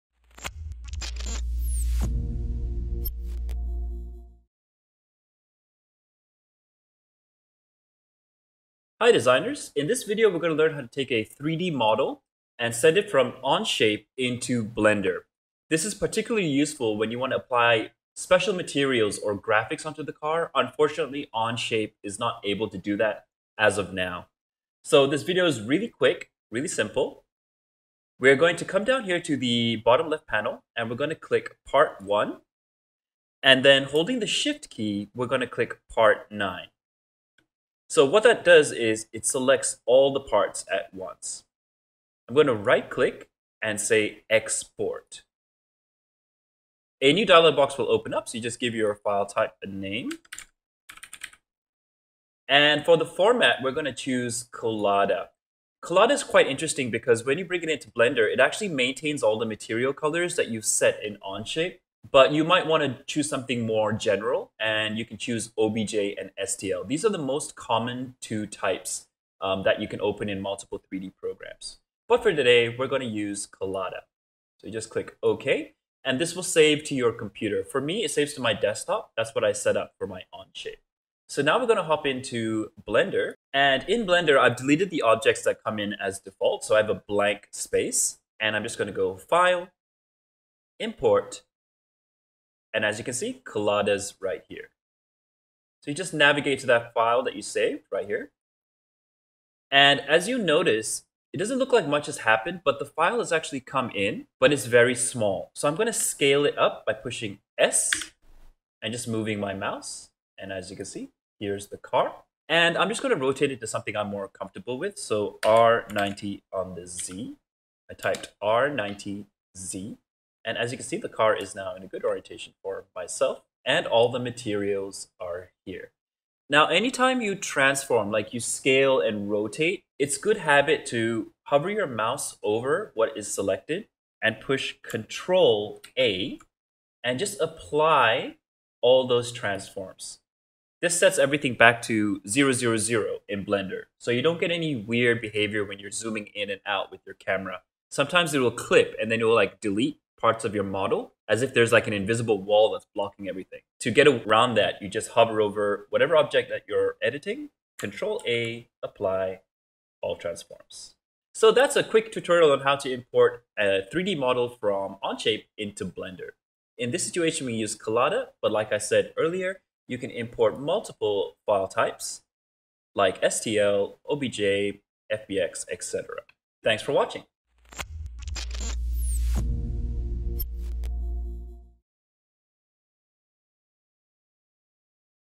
Blender Basics 1.6 - Exporting from Onshape to Blender!
In this quick tutorial we will learn how to export a 3D model from Onshape into Blender!

Please make sure you are using the most updated stable Blender version before following along with this tutorial: